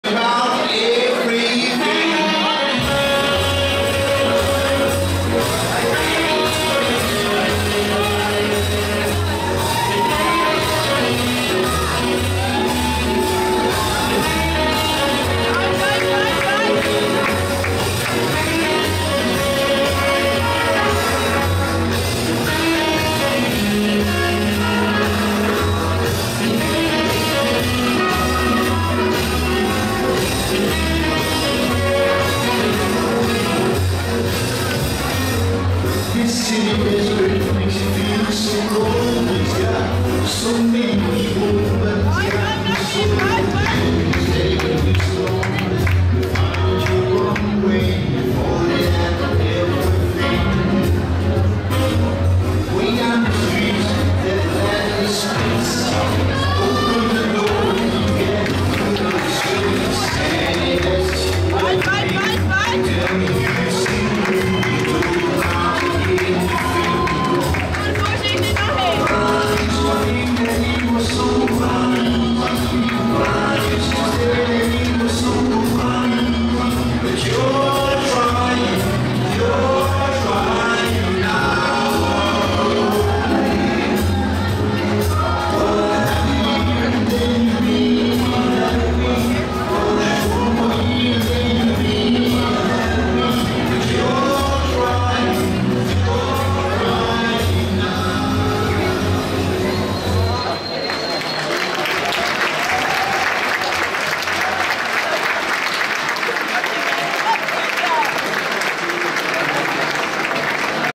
MČR STT 2023, Zárybnický - Hegrová, 1. kolo, Slowfox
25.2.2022
Mistrovství České republiky standardních tanců 2022
Dospělí, 1. kolo
Praha

Jakub Zárybnický - Veronika Hegrová